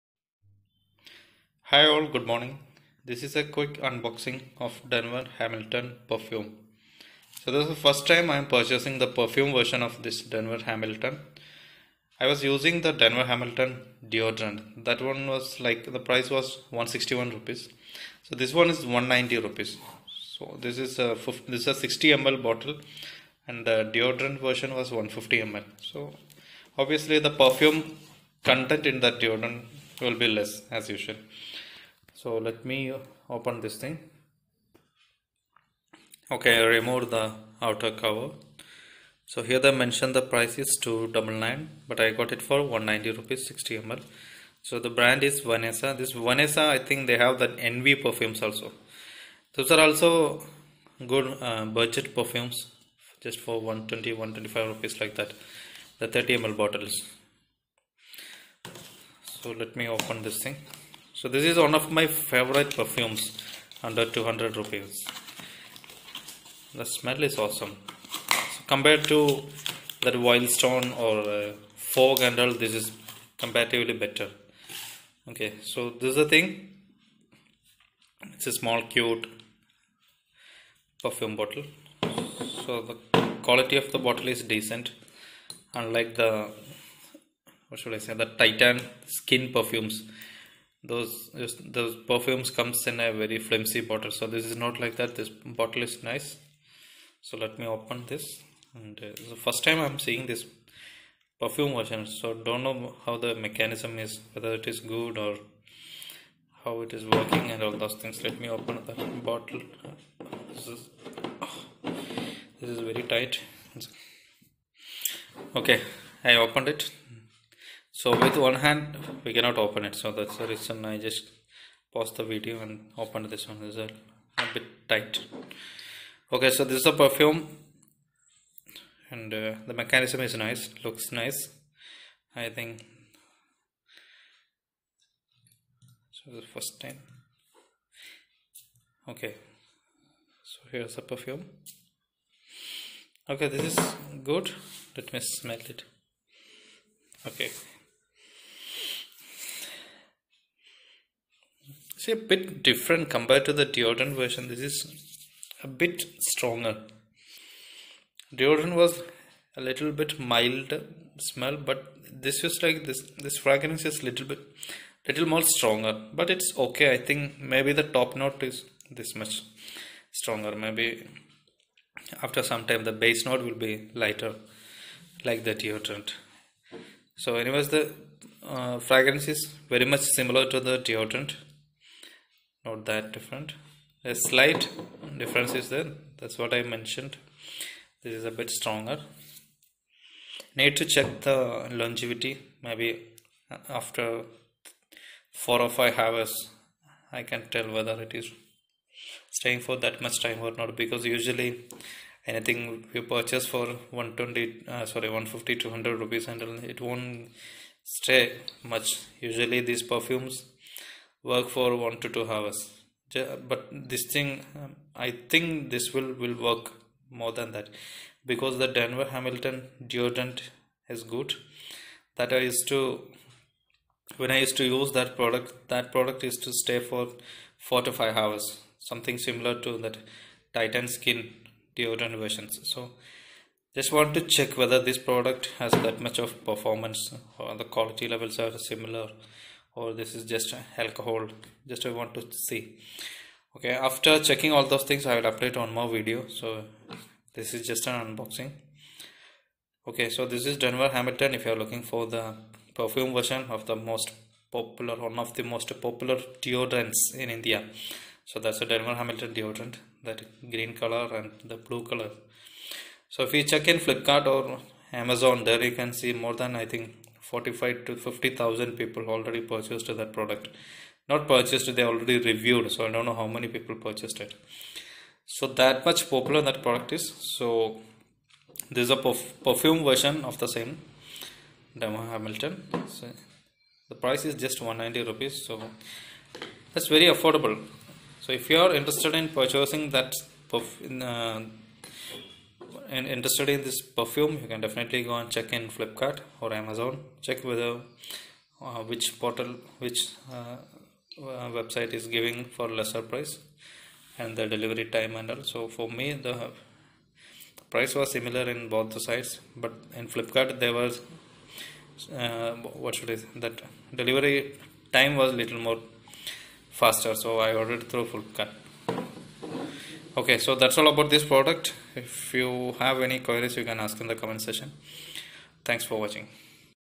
Denver Hamilton Perfume
This is a quick unboxing of Denver Hamilton, one of the most popular perfumes in India.